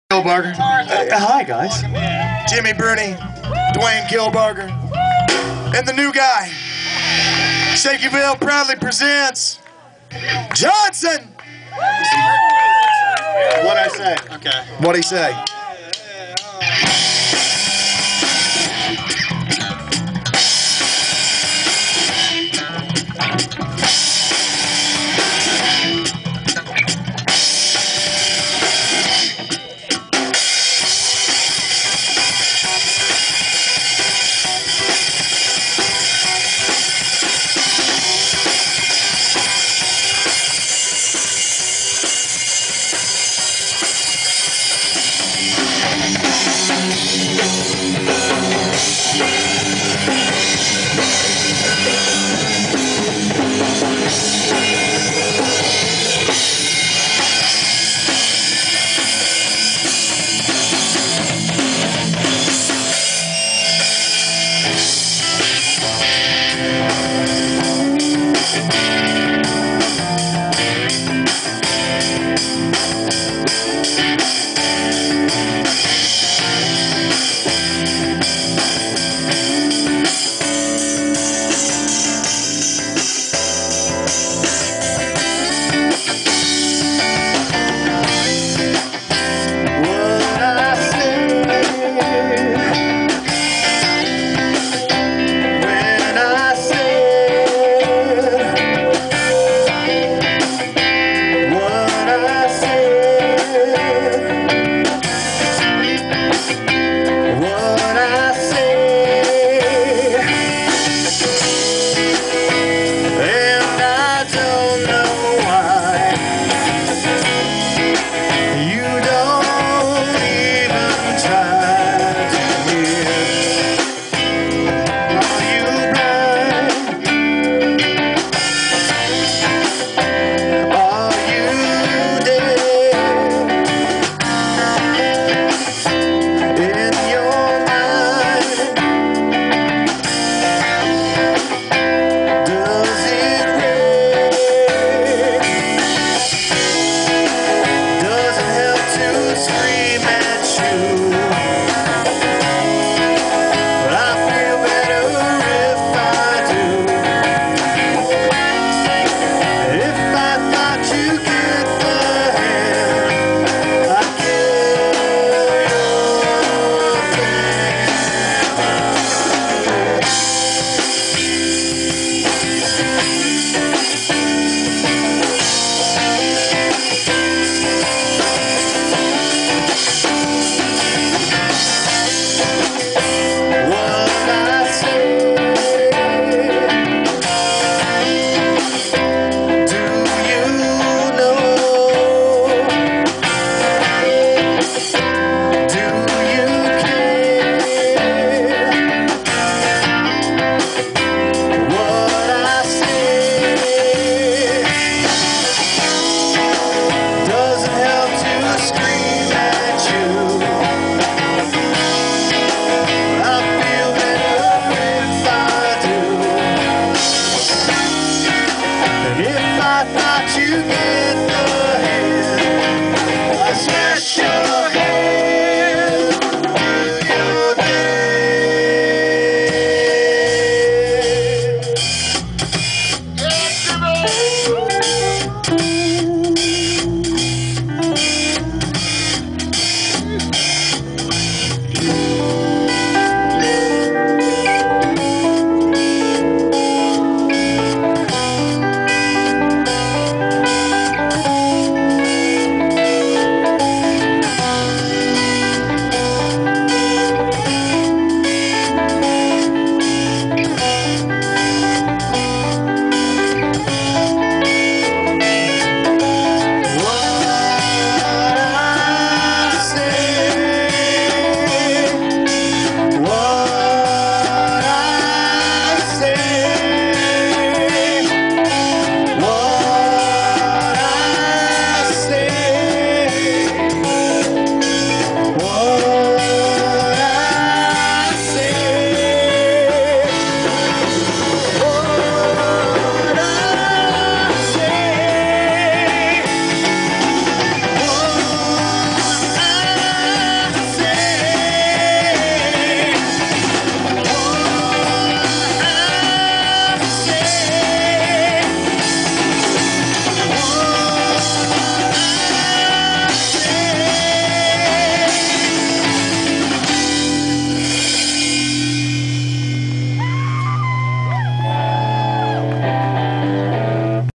Johnson - Shakyville, 2009
Johnson reunion show at Shakyville 2009 North of Somerset, Ohio.  Kenny Smith, the guitarist on the left, used to play with me in Red Dahlia.